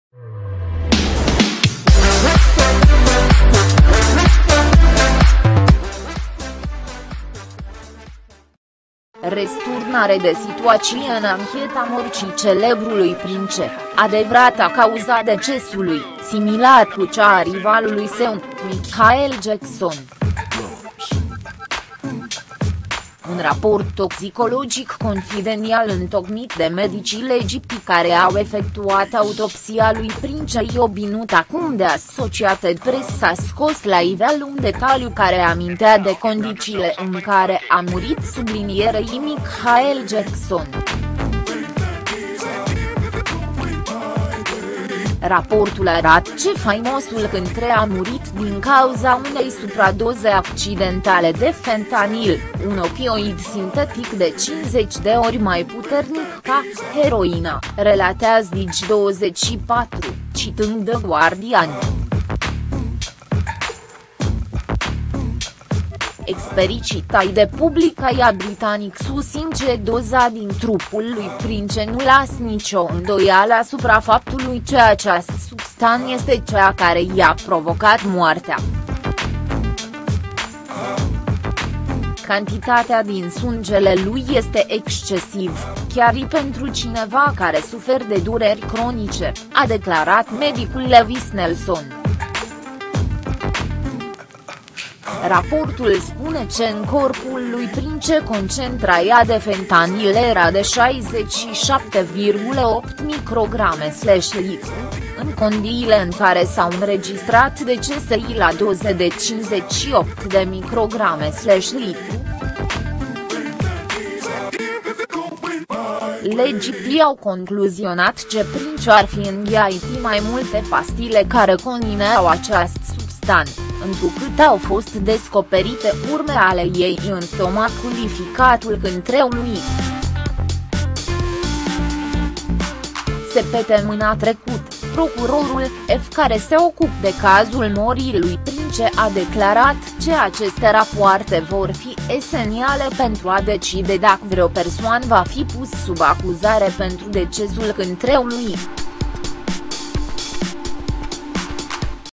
Răsturnare de situaţie în ancheta morţii celebrului Prince: Adevărata cauză a decesului,
Răsturnare de situaţie în ancheta morţii celebrului Prince: Adevărata cauză a decesului, similară cu cea a rivalului său, Michael Jackson